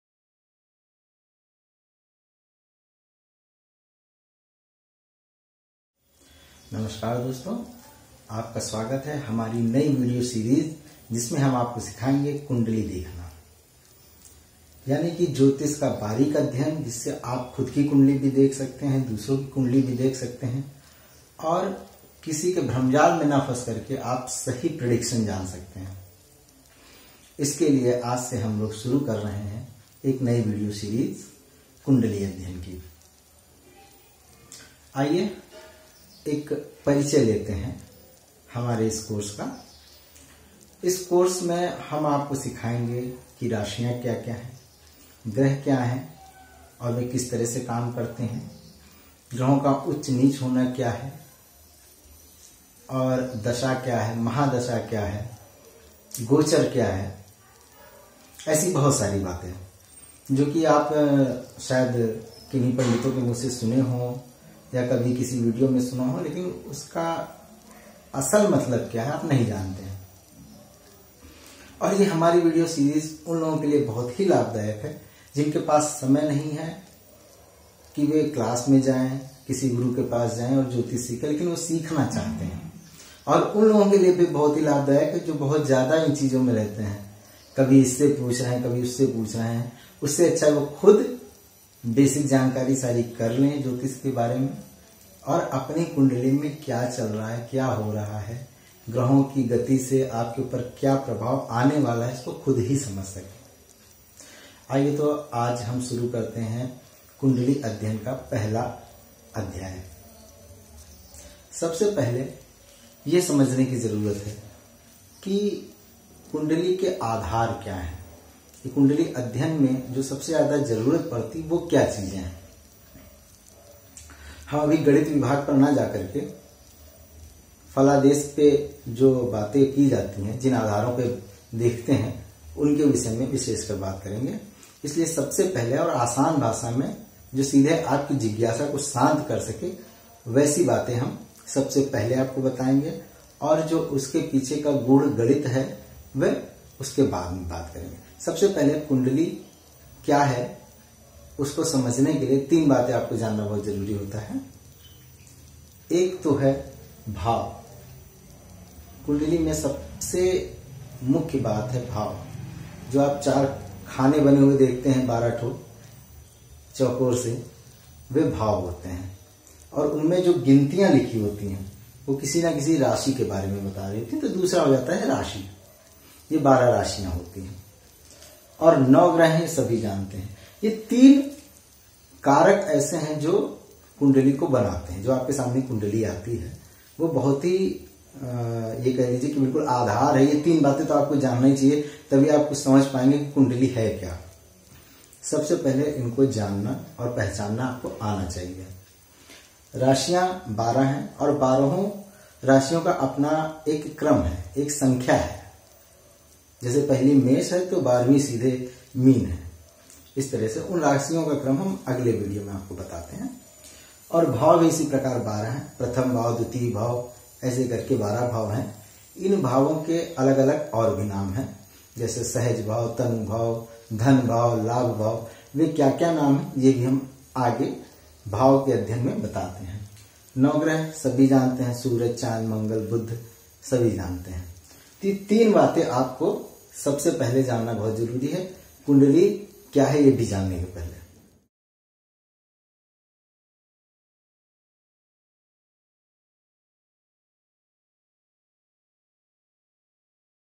Jyotish basics part-1
कुण्डली अध्ययन के आधारभूत सिद्धान्तों कप परिचय।